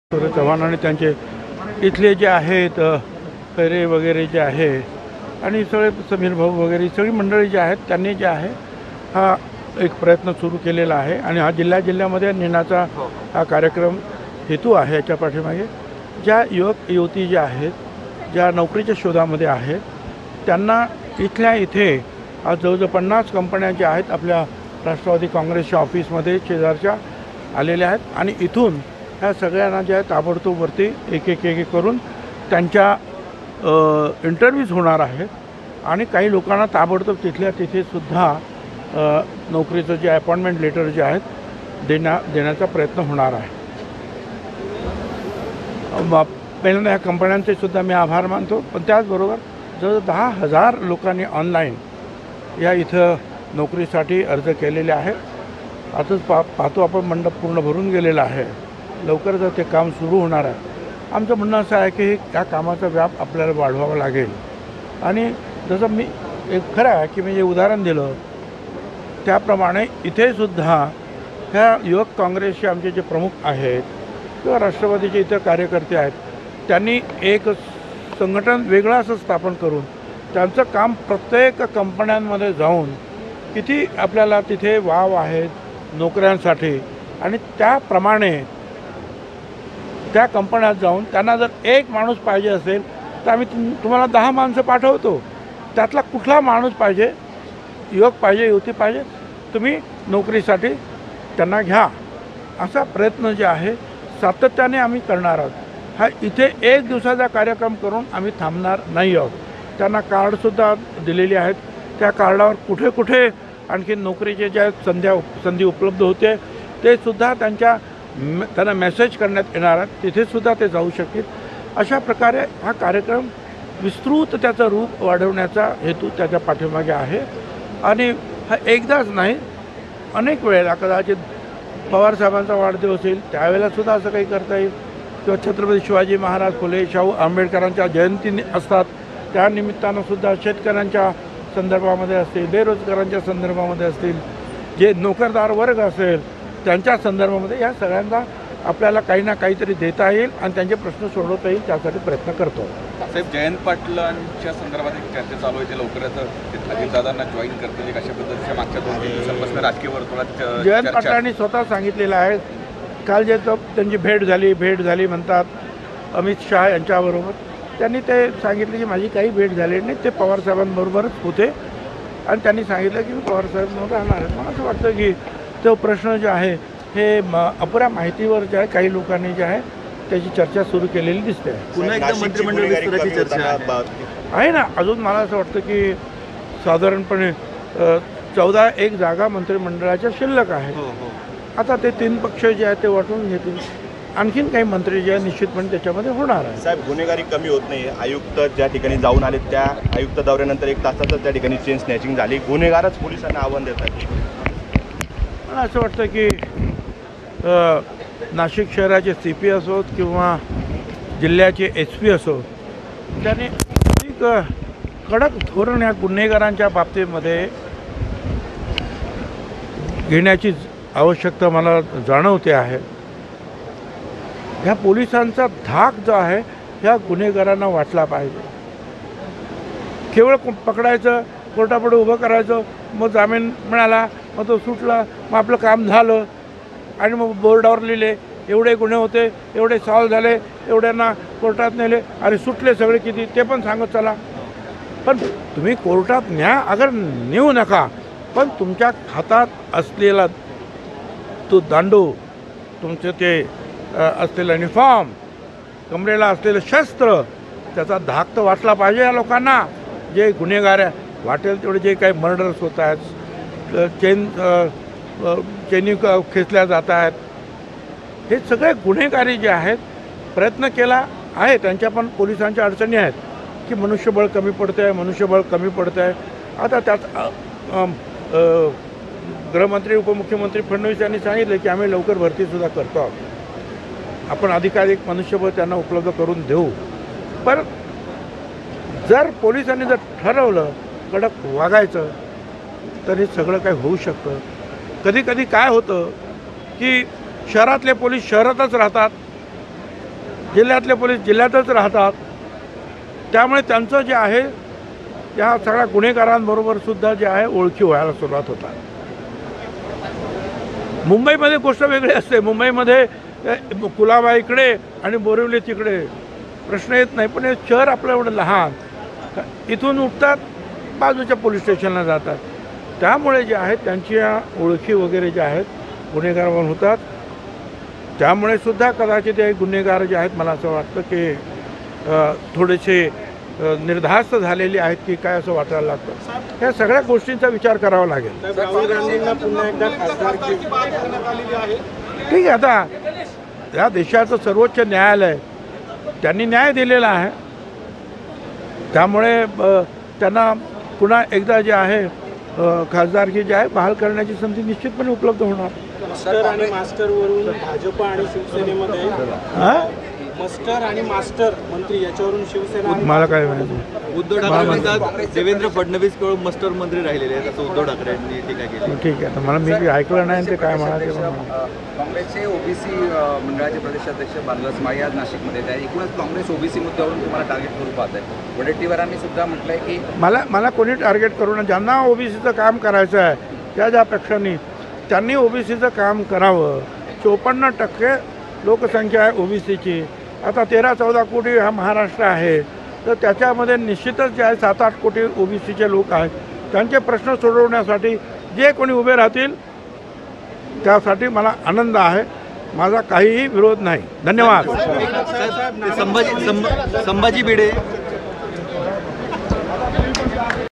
NASHIK | ओबीसींच्या प्रश्नासाठी जे पक्ष काम करतील त्याला माझा विरोध नाही - छगन भुजबळ
ऑन रोजगार महोत्सव
- अजित पवार यांच्या वाढदिवसानिमित्त हा कार्यक्रम घेत आहोत
- युवक युवती आहे त्यांना नोकऱ्या देण्याचा प्रयत्न आहे.
- काही लोकांना ताबडतोब नोकरी देण्याचा प्रयत्न आहे
- 10 हजार लोकांनी ऑनलाइन अर्ज केले आहेत.
- मी एक उदाहरण दिलं हे खरं आहे
- एक कमिटी स्थापन करून कंपनीत जाऊन माहिती घ्यावी
- 1 जागा पाहिजे असेल तर 10 लोक पाठवून प्रयत्न केला पाहिजे.
- एक कार्यक्रम घेऊन आम्ही थांबणार नाही
- शरद पवार  यांचा  वाढदिवस आला तेव्हाही असा कार्यक्रम घेऊ
- शिवाजी महाराज यांच्या जयंतीला घेऊ

ऑन जयंत पाटील
-अमित शहा यांच्या बरोबर भेट झाली म्हणतात,त्यांनी सांगितले आहे
 त्यांची भेट झाली नाही

ऑन मंत्रिमंडळ विस्तार
- 14 जागा शिल्लक आहे
- वरती निर्णय होईल

ऑन पोलीस गुन्हेगारी
- पोलिसांचा धाक गुन्हेगारांना वाटला पाहिजे
- कोर्टापुढे उभे करून काम झालं असे नाही
- जामीन झाला की संपले असे नाही
- खाकीचा आणि दंडुक्याचा धाक राहिला पाहिजे
- पोलिसांच्या अडचणी आहेत
- मनुष्यबळ कमी पडत आहे
- त्याबद्दल निर्णय घेतला जाईल
- शहरातील पोलीस शहरात राहतात आणि जिल्ह्यातले जिल्ह्यात राहतात
- वर्षानुवर्षे पोलीस त्याच शहरात असल्याने गुन्हेगार आणि पोलिसांची ओळख वाढते
- मुंबईत तसे नाही

ऑन राहुल गांधी
- सर्वोच्च न्यायालयाने न्याय दिला आहे
- पुन्हा ते खासदार झालेत

ऑन ओबीसी
- ज्यांना ओबीसीचे काम करायचे आहे त्यांनी करावे
- ओबीसींच्या प्रश्नासाठी जे पक्ष काम करतील त्याचा आनंद आहे
- माझा कुणाला ही विरोध नाही

BYTE : अन्न व नागरी पुरवठा मंत्री छगन भुजबळ